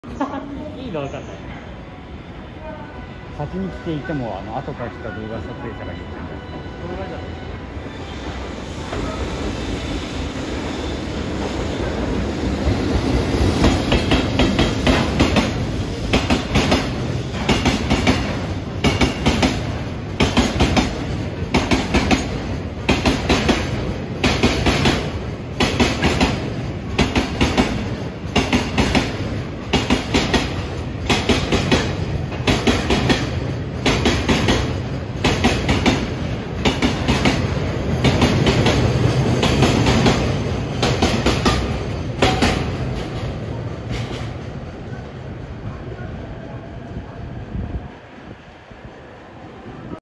【ジョイント音最高！】国鉄EF65形2000番台2096号機（赤プレート・国鉄色）＋タキ20B（両）が大宮駅10番線を通過するシーン（臨8685レ・新A55・石油輸送）2023.1.30
前にいたおっさんがスマホ鉄をめちゃくちゃ聞こえるように批判しててその音声が冒頭入ってるかもです。
大宮自治との戦いは続く。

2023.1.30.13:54